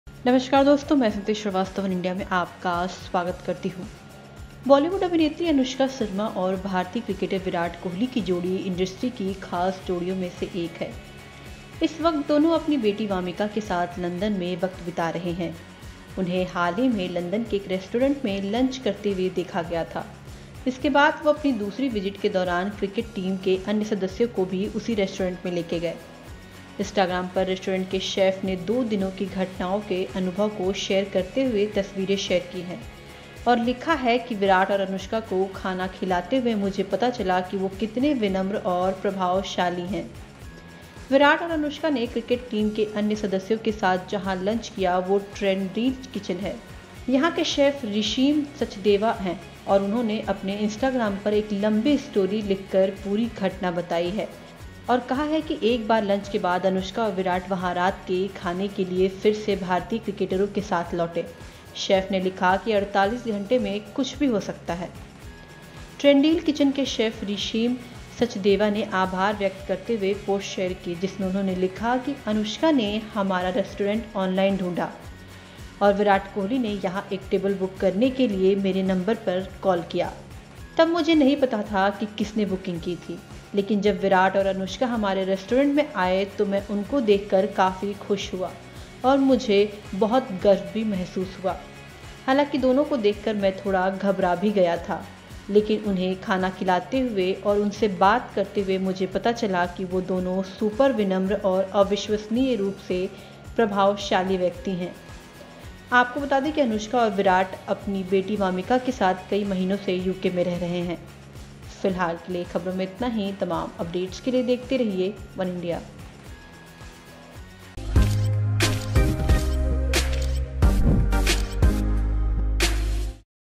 Anushka Sharma & Virat Kohli are back to this Vegan Restaurant in England, See Pics | वनइंडिया हिंदी
Anushka Sharma is always on the lookout to explore new vegan food items wherever she travels to. The actress who is currently in England with her husband Virat Kohli recently enjoyed a vegan meal at the Tendril Kitchen restaurant in England. Looks like the couple loved the food that they had at the restaurant as they headed there again but this time with some of their friends.

बॉलीवुड अभिनेत्री Anushka Sharma और भारतीय क्रिकेटर Virat Kohli की जोड़ी इंडस्ट्री की खास जोड़ियों में से एक है। इस वक्त दोनों अपनी बेटी Vamika के साथ London में वक्त बिता रहे हैं। उन्हें हाल ही में लंदन के Tendril Kitchen restaurant में लंच करते हुए देखा गया था। इसके बाद वो अपनी दूसरी विजिट के दौरान क्रिकेट टीम के अन्य सदस्यों को भी उसी रेस्तरां में ले गए।